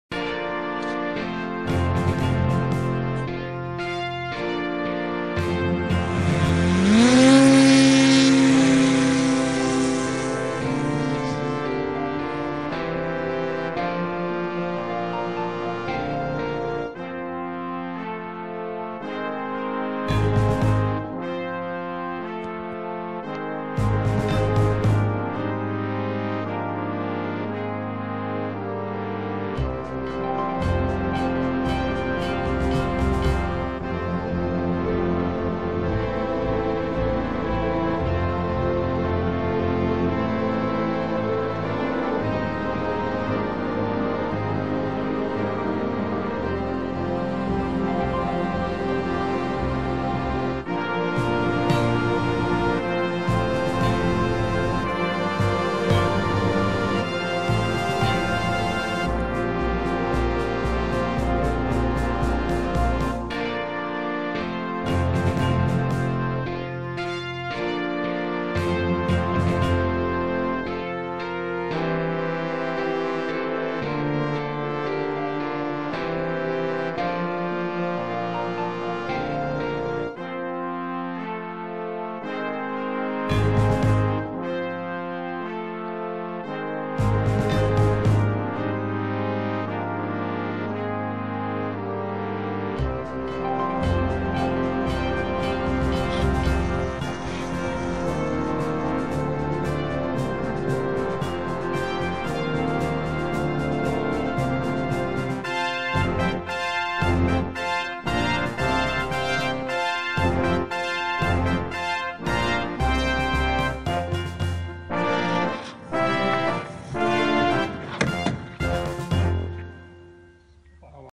K8 Phönix Modellbau (neueste Anschaffung)
Das ist das neustes Modell, anstatt der Sopwith Pup die verkauft wurde. Reizvoll der eigene E-Antrieb zu der Schleppkupplung dazu. Mächtig mit einer Spannweite von 6.000 mm.